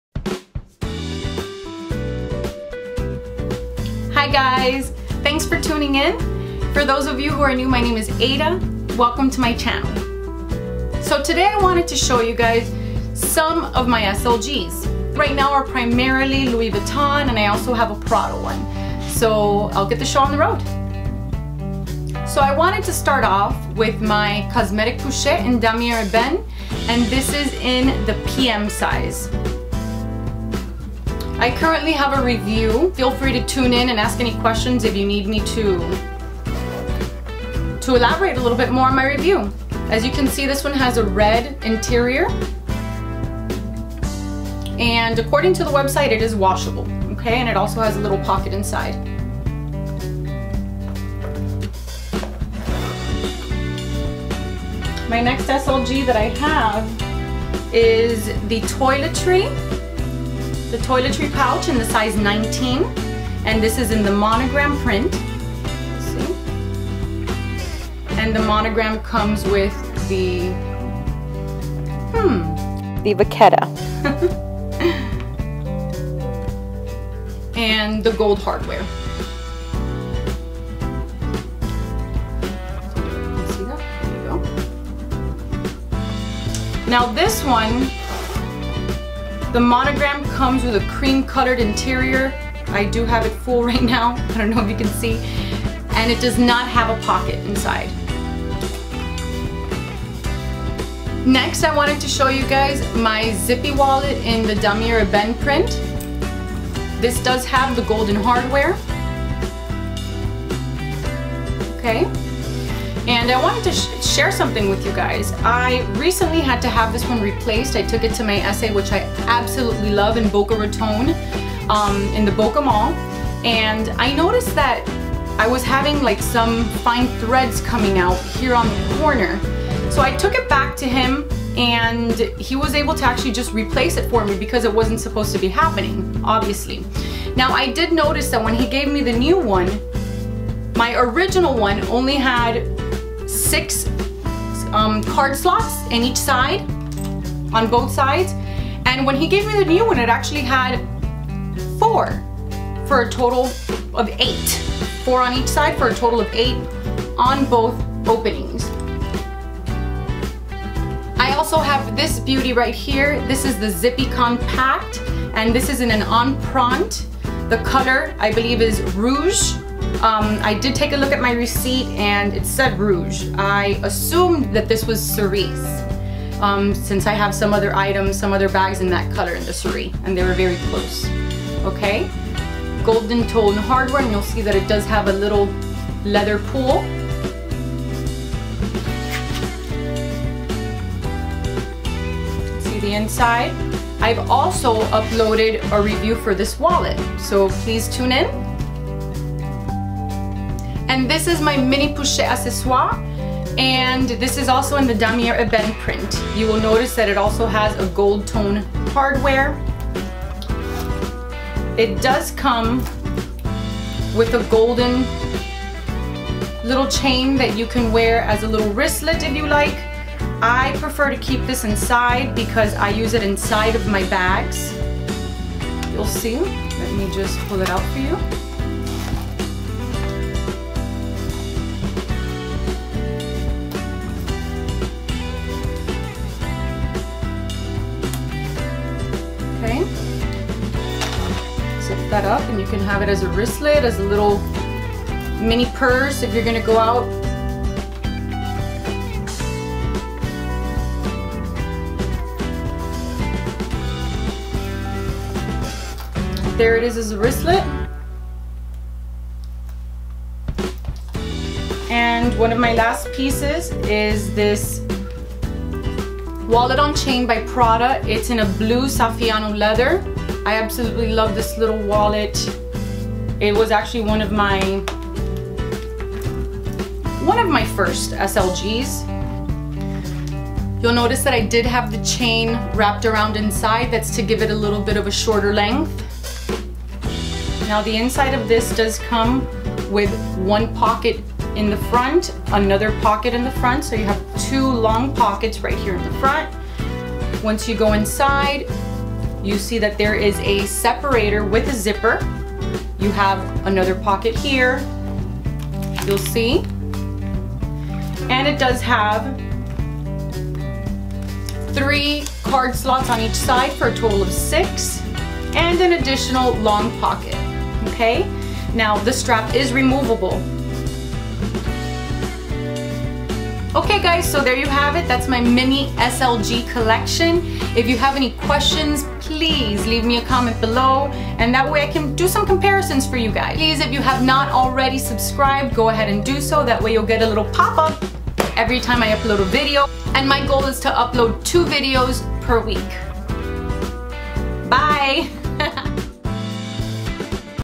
My Growing SLG's Collection 2017
WRITE ME!
USA

Items mentioned this video:

1. Louis Vuitton Mini Pochette Accessories
2. Louis Vuitton Toiletry 19
3. Louis Vuitton Cosmetic Case
4. Louis Vuitton Zippy Wallet
5. Louis Vuitton Zippy Compact Wallet